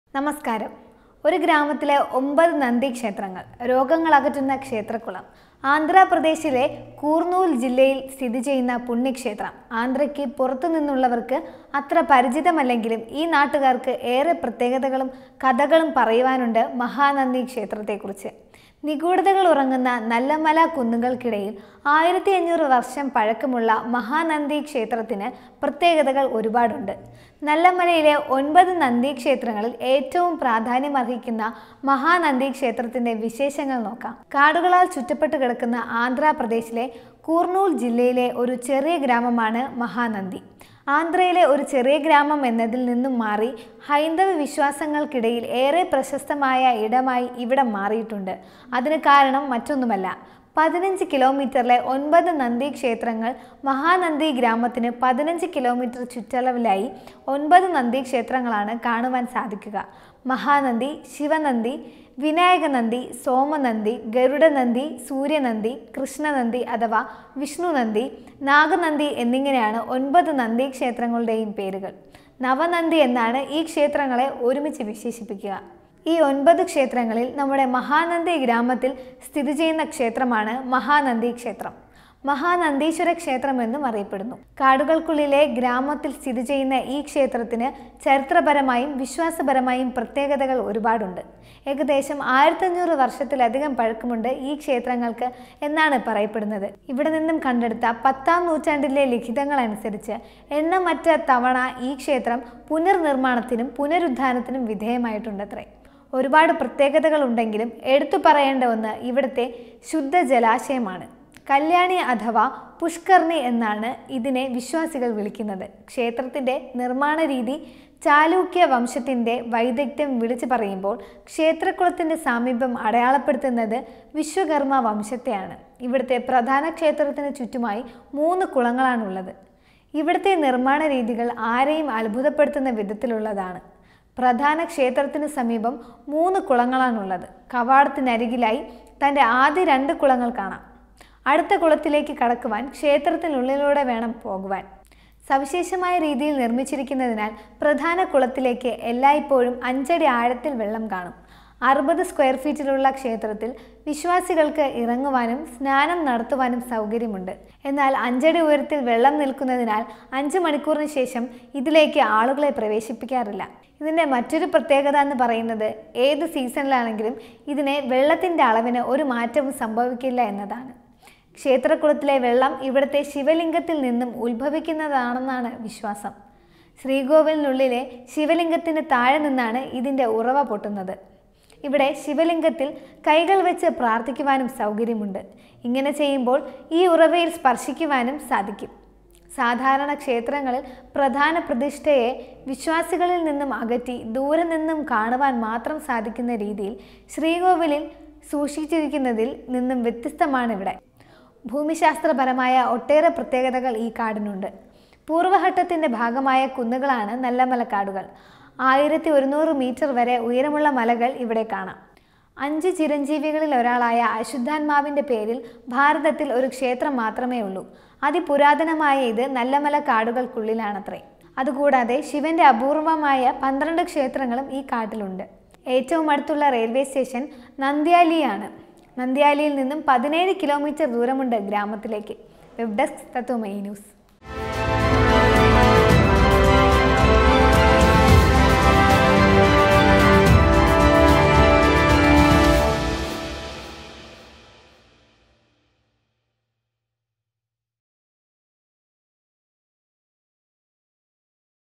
15 കിലോമീറ്ററിനുള്ളിൽ 9 നന്ദി ക്ഷേത്രങ്ങളുള്ള ഗ്രാമം | MAHA NANDI TEMPLE
15 കിലോമീറ്ററിനുള്ളിൽ  9 നന്ദി ക്ഷേത്രങ്ങളുള്ള ഗ്രാമം | NANDI TEMPLE |